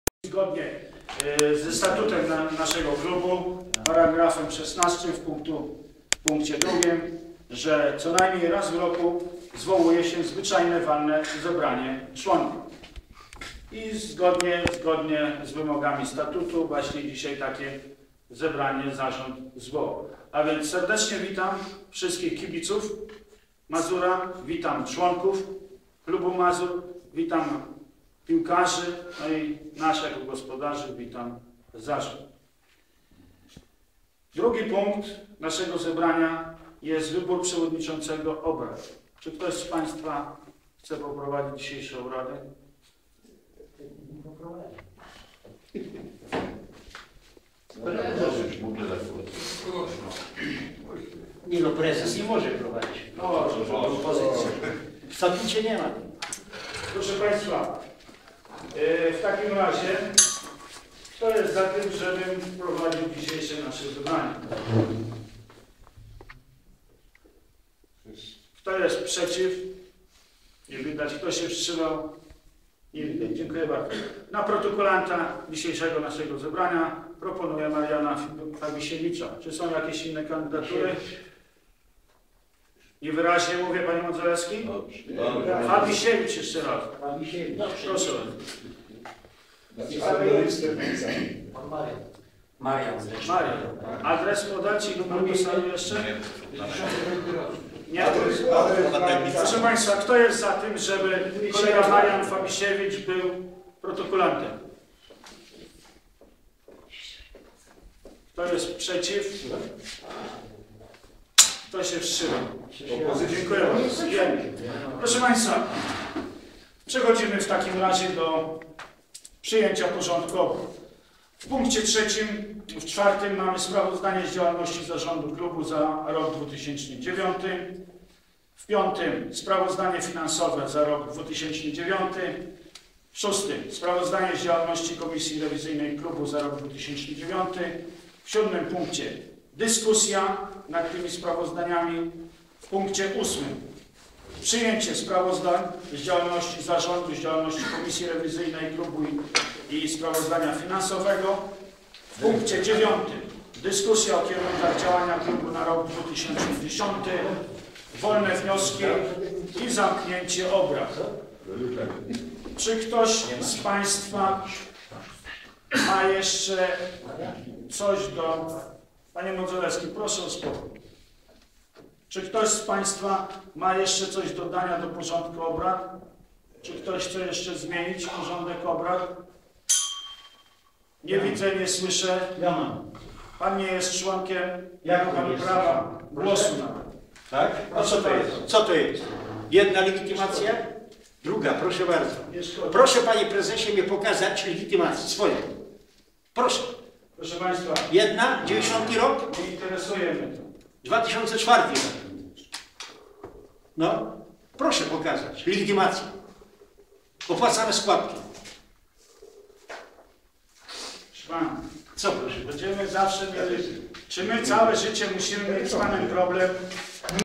Burzliwe zebranie w Mazurze
Utarczki słowne pomiędzy obecnym prezesem Mazura Krzysztofem Rumińskim, a byłym sternikiem klubu Zenonem Modzelewskim zdominowały czwartkowe spotkanie sprawozdawcze piskiego ligowca.